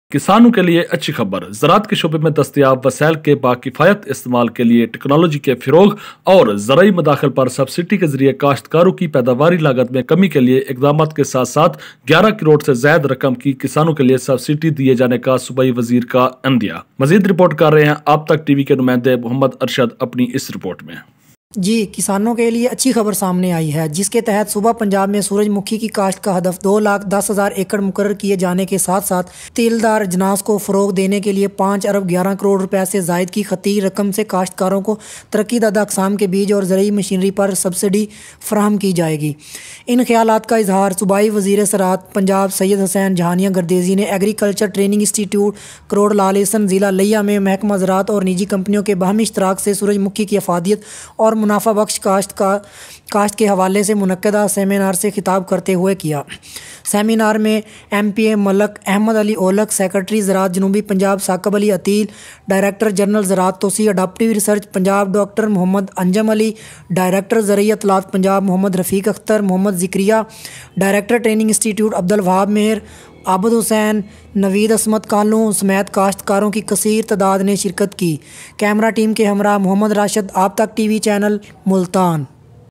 Multan: Good news for farmers. Subsidy for farmers of more than 11 crore I Aap Tak Tv Channel
ملتان :کسانوں کےلیے اچھی خبر11کروڑ سے زائد رقم کی کسانوں کےلیے سبسڈی تفصیلات اس ویڈیو میں AAP TAK TV